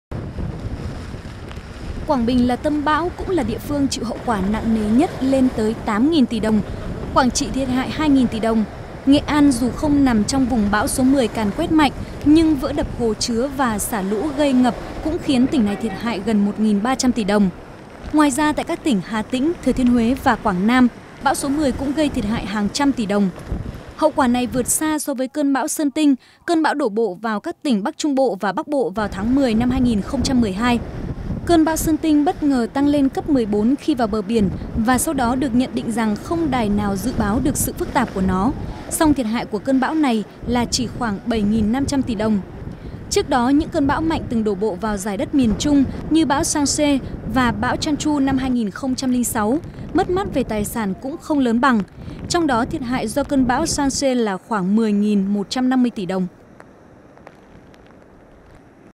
VTC14_Siêu bão số 10 gây thiệt hại lớn nhất về tài sản trong hàng chục năm qua_06.10.2013
Siêu bão số 10 gây thiệt hại lớn nhất về tài sản trong hàng chục năm qua:
Theo tổng hợp từ các địa phương, siêu bão số 10 năm 2013 với tên quốc tế là Wutip, đã gây thiệt hại lên tới gần 11 nghìn tỷ đồng. Đây không chỉ là cơn bão mạnh nhất mà còn là cơn bão gây thiệt hại lớn nhất về tài sản trong hàng chục năm qua.